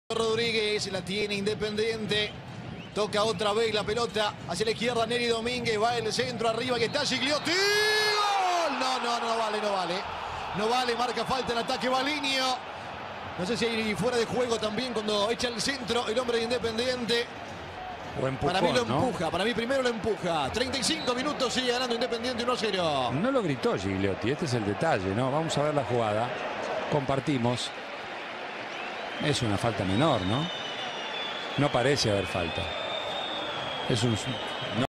IRTV Fecha 22 | El insólito gol anulado a Emmanuel Gigliotti
A Emmanuel Gigliotti le anularon el gol que hubiese sido el 2-0 ¿Qué cobró el árbitro?
InfiernoRojo.com